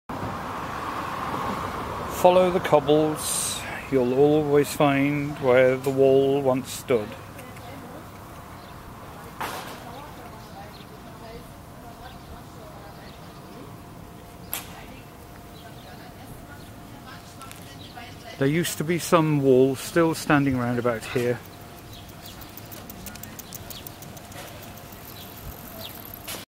07 Follow the cobbles
In 1990 my late partner and I walked from Checkpoint Charlie to the Ostbahnhof following our noses and a map. Parts of the wall had already disappeared. We only had instinct and map reading skills to guide us.

In 2013, I followed the walk with only my memory, a few photos and the video camera to register what once was.